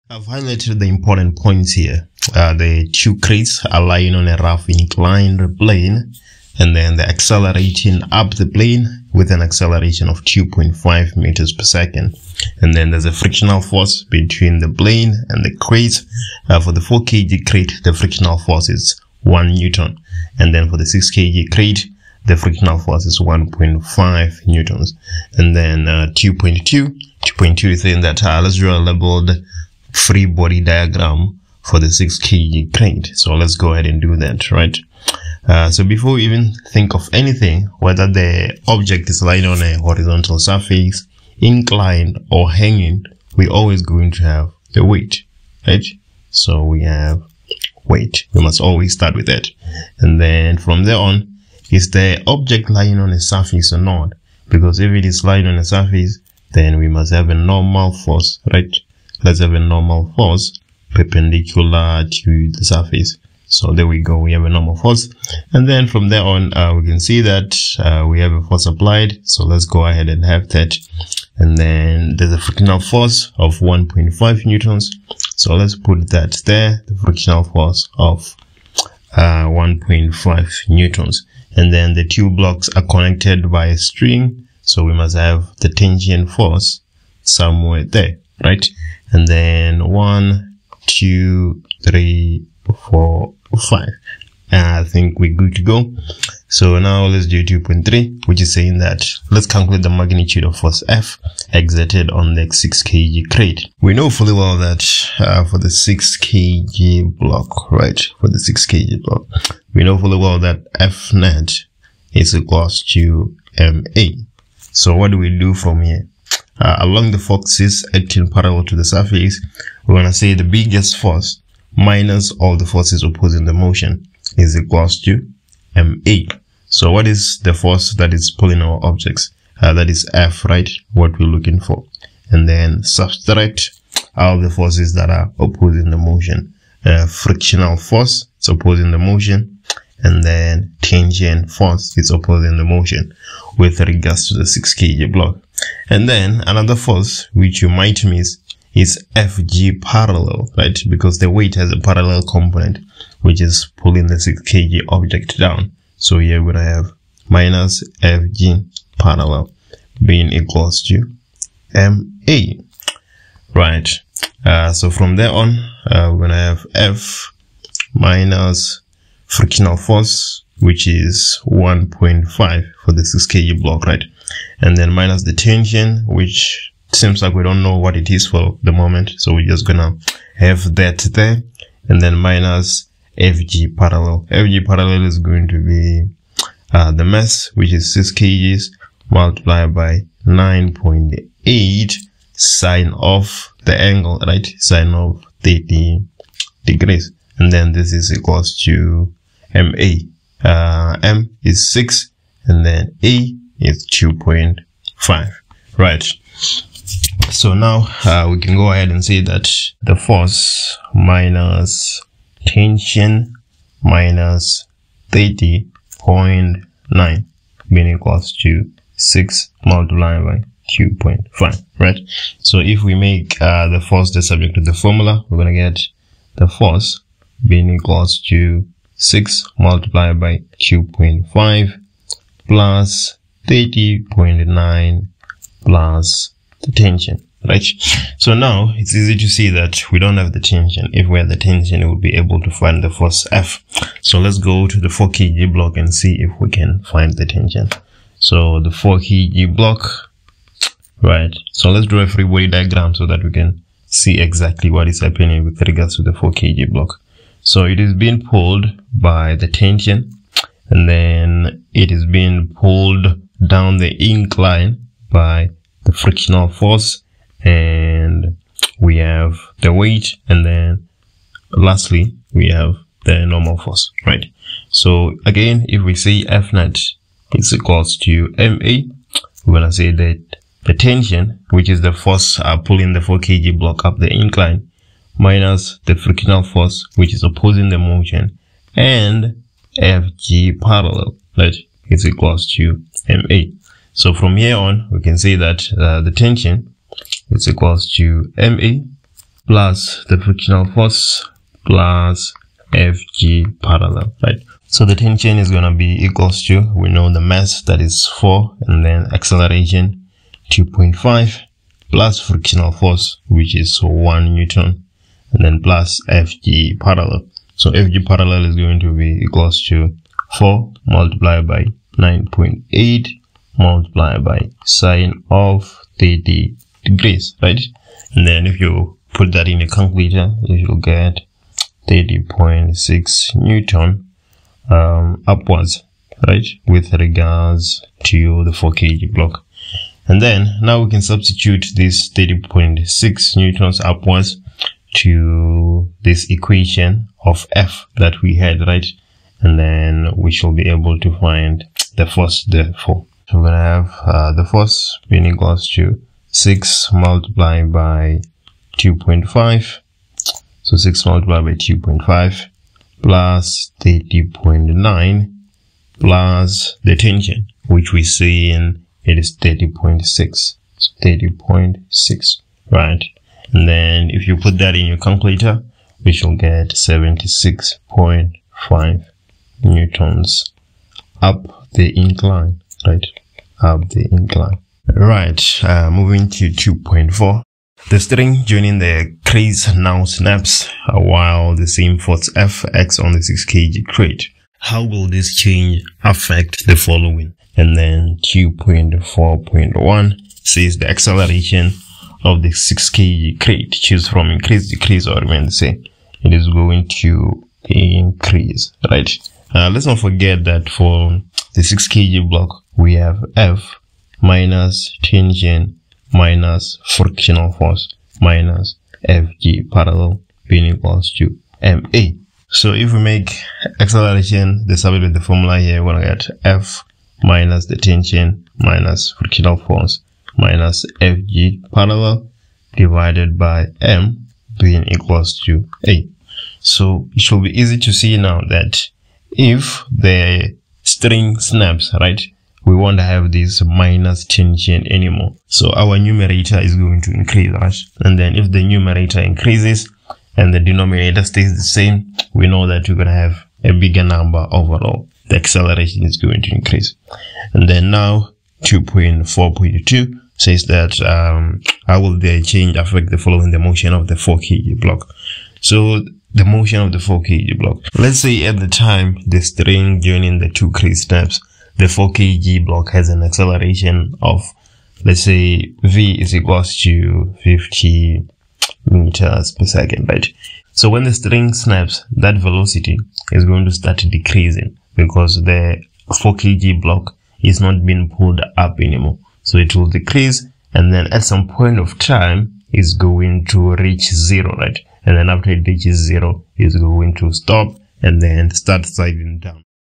Newton's Laws of Motion | Grade 12 Math & Sciene
Question 2 Grade 12 Physics P1 September Trial Exam | Newtons Laws of Motion